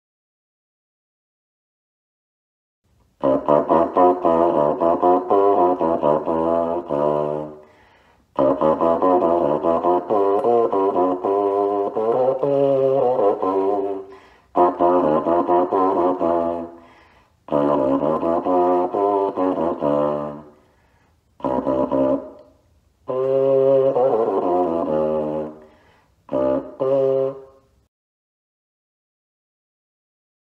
A two-foot flat-form pocket serpent horn
A two-foot flat-form pocket serpent horn by Scott F. Hall in cut and laminated wood--played here with a three-inch polyvinyl chloride tube bocal inserted and connected to a small-shank trombone mouthpiece (6.5 AL). This particular instrument is in the collection of Oscar Abella, principal tuba player with the Basque National Orchestra and resident of Hendaye, France.